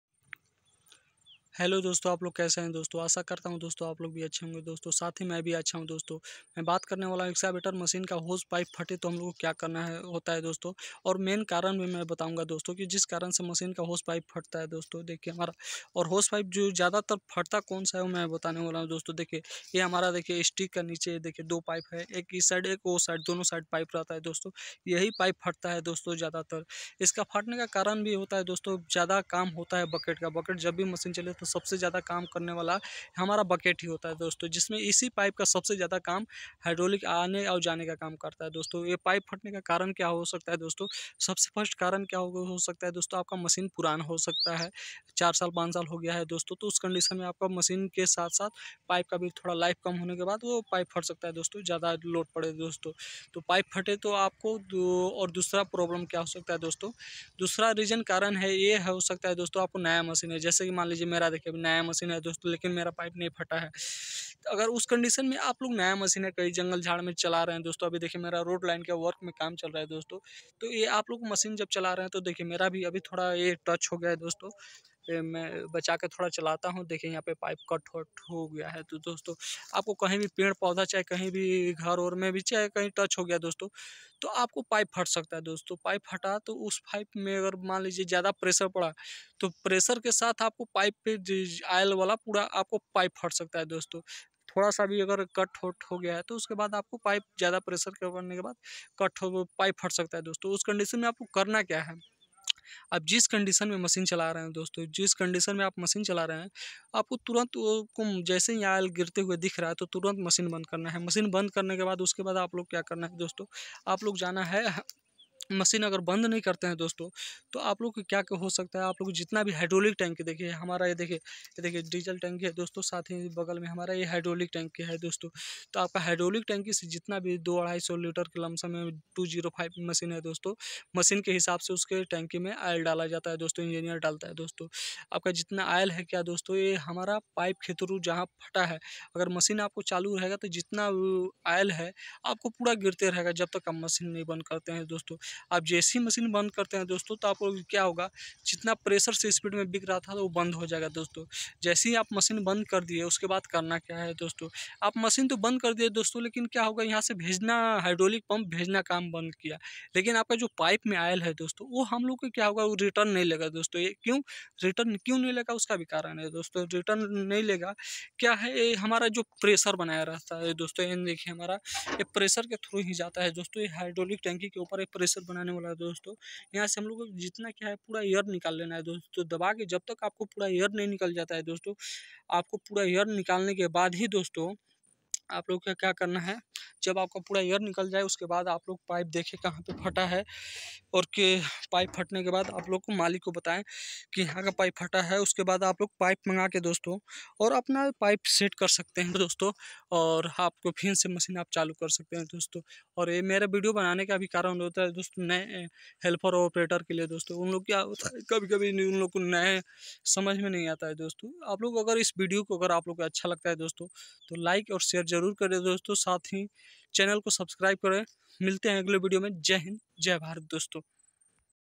exavator machine ki hos pipe bhat Jaye to kya kare
exavator machine hos pipe bhate to pahale 2 kam kareExavator machine ki hos pipe bhat jaye to kya karen